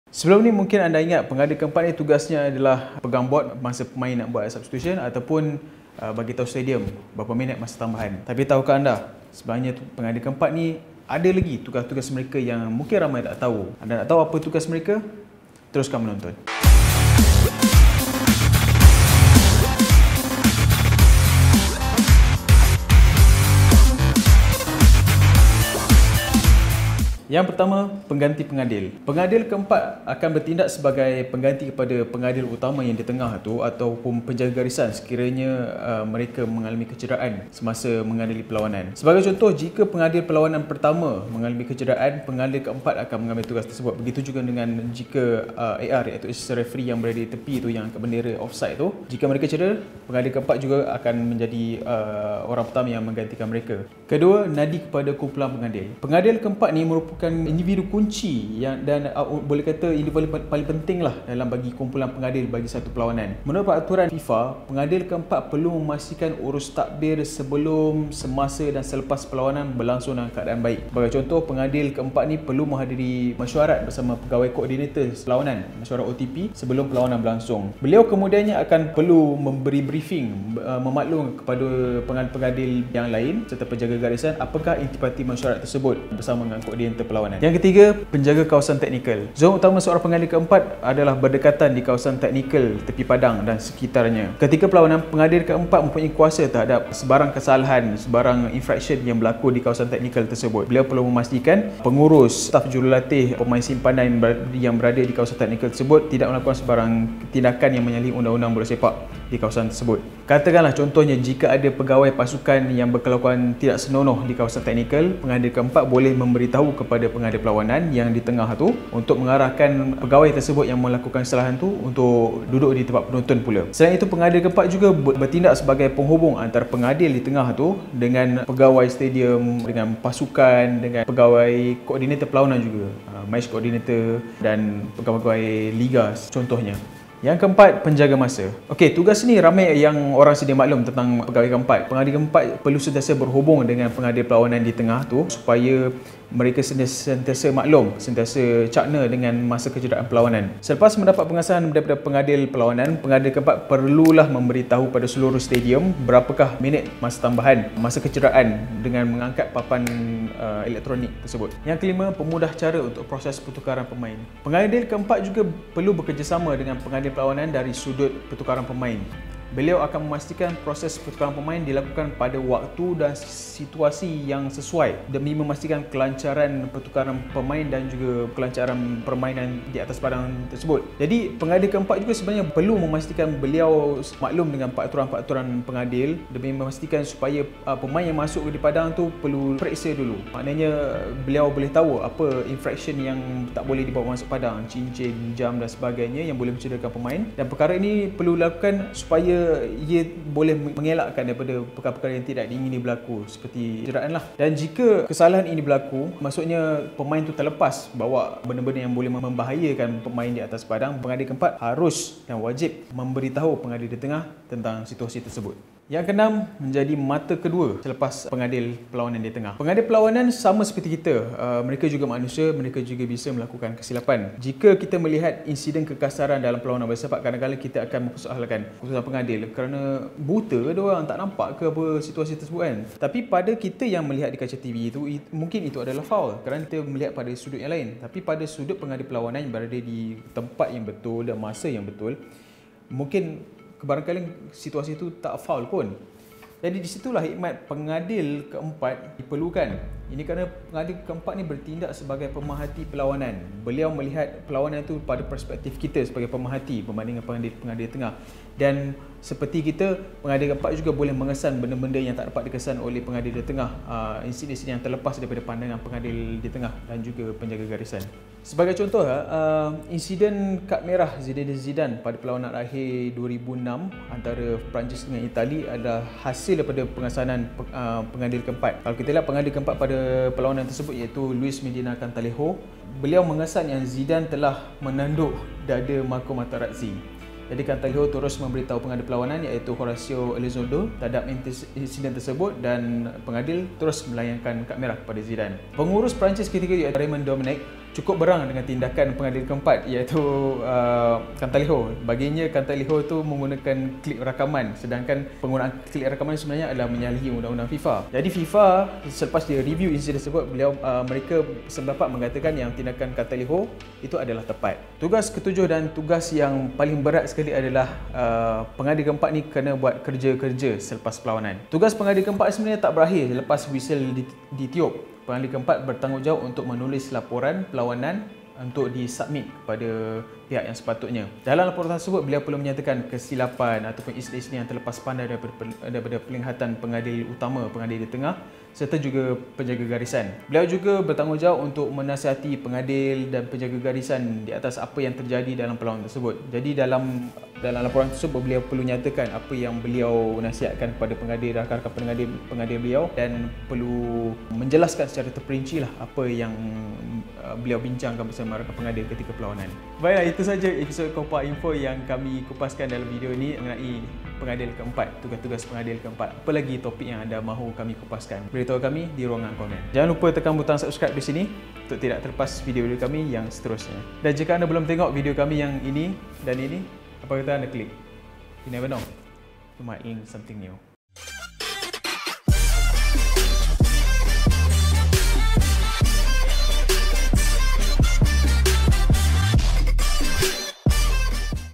Tugas Pengadil Ke-4 Yang Perlu Anda Tahu
Sebelum ni, mungkin anda ingat pengadil ke-4 ni tugasnya pegang board number pemain masa nak buat substitution dan bagitahu berapa minit masa tambahan. Tapi tahukah anda tugas pengadil ke-4 ni lagi banyak dan sama berat sebenarnya dengan pengadil lain? Kalau anda nak tahu apa lagi tugas mereka, teruskan menonton.